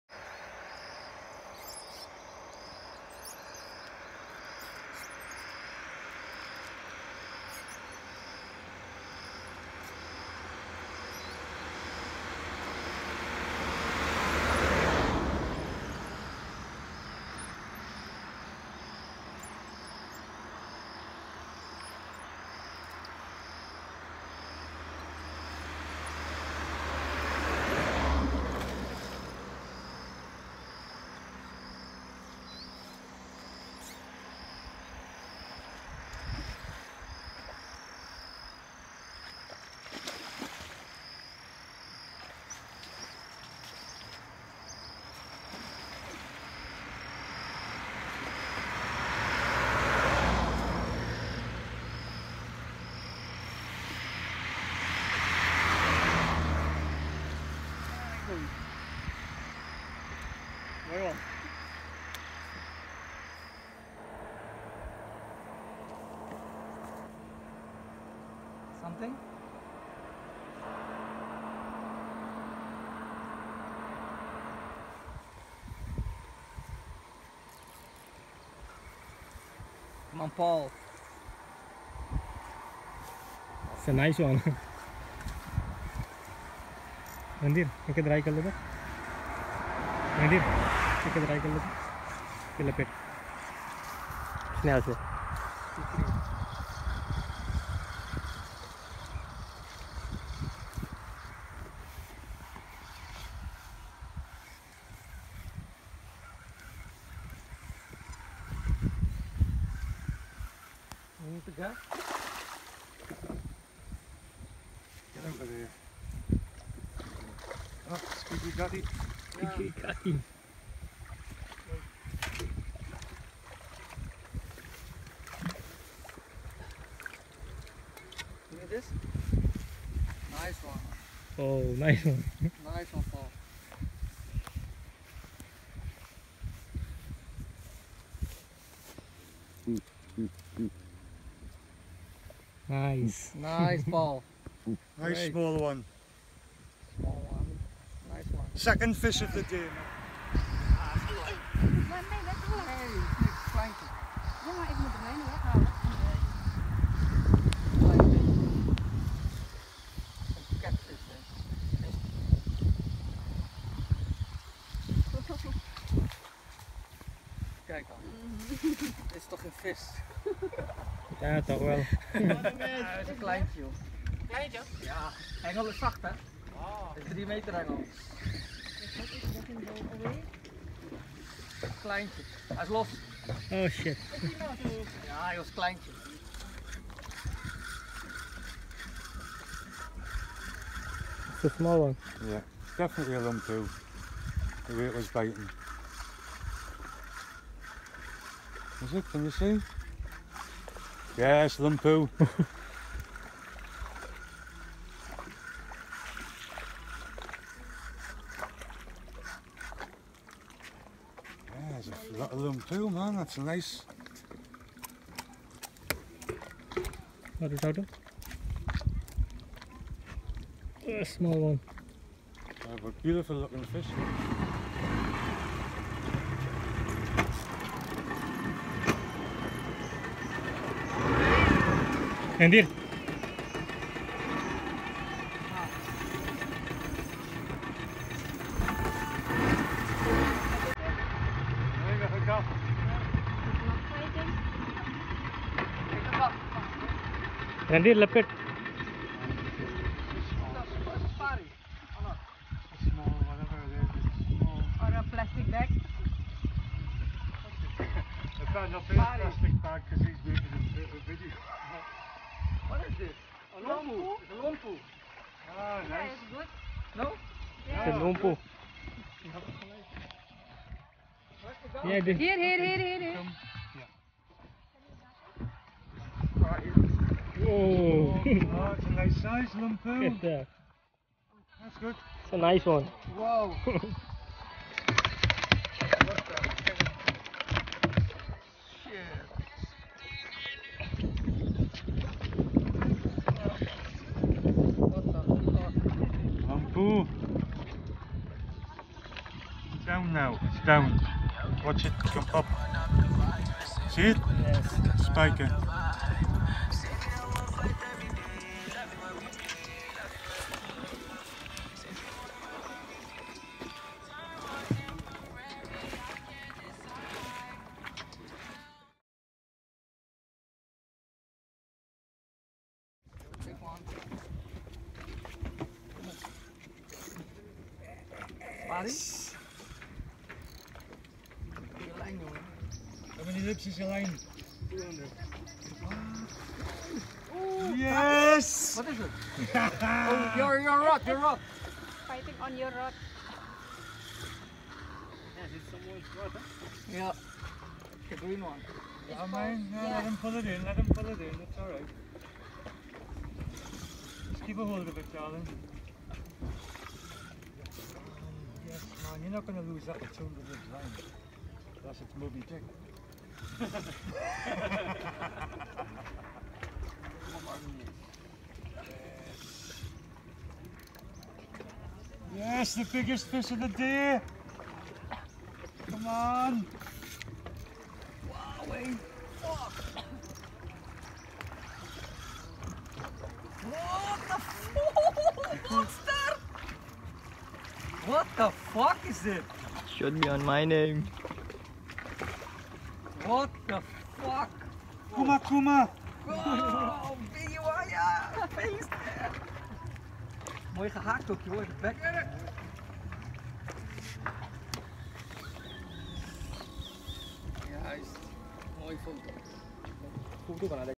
Fishing for Catfish in Suriname
Catfish hunting
Types of catfish caught: Tiger shovelnose catfish, Kuma kuma catfish and Oyster toad fish